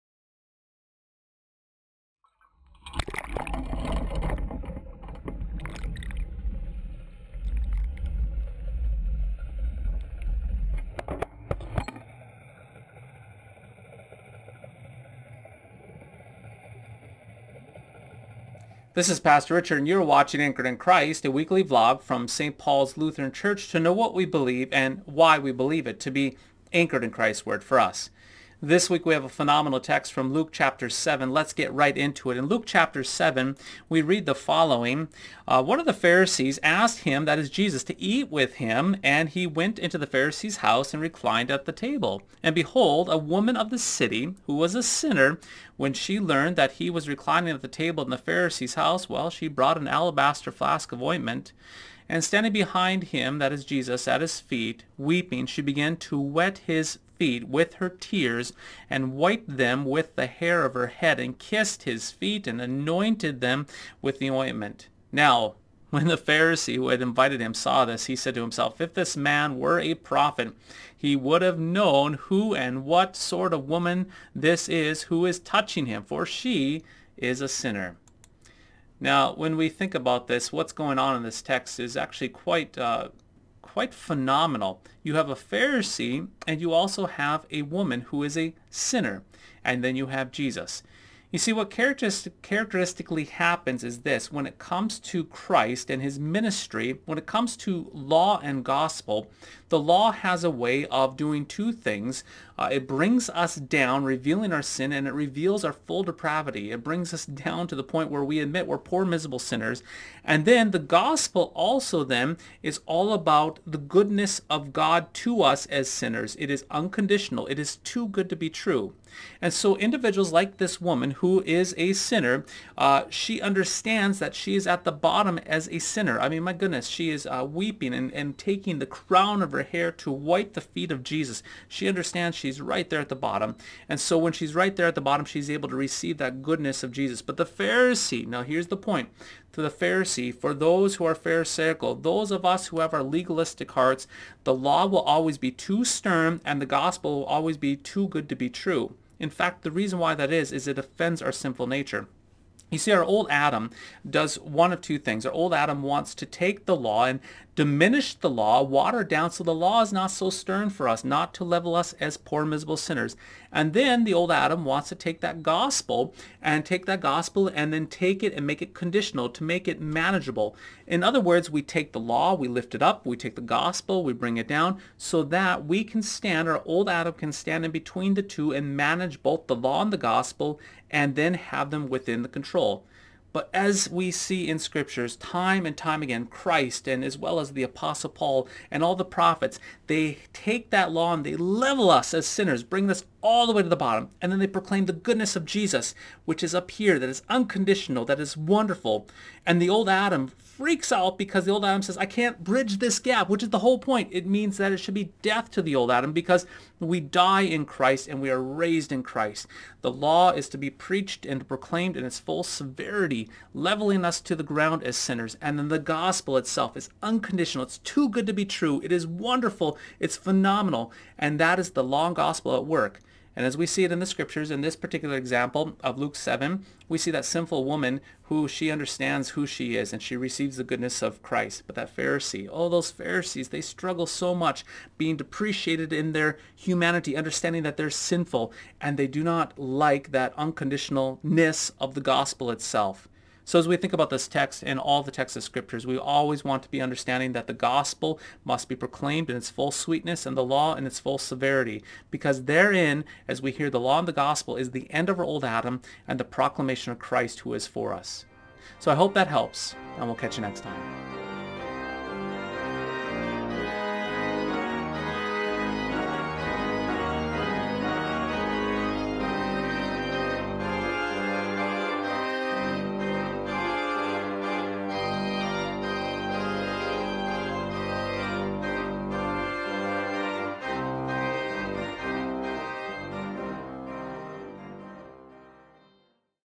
Anchored In Christ 031 | The Law and Gospel of Foot Washing
When the woman with the alabaster jar washes Jesus' feet, a lesson in Law and Gospel is already in the making.

Crucial Productions - Teaching you Christianity so you can pass it

Or just open up your favorite podcast app and search for “Crucial Productions”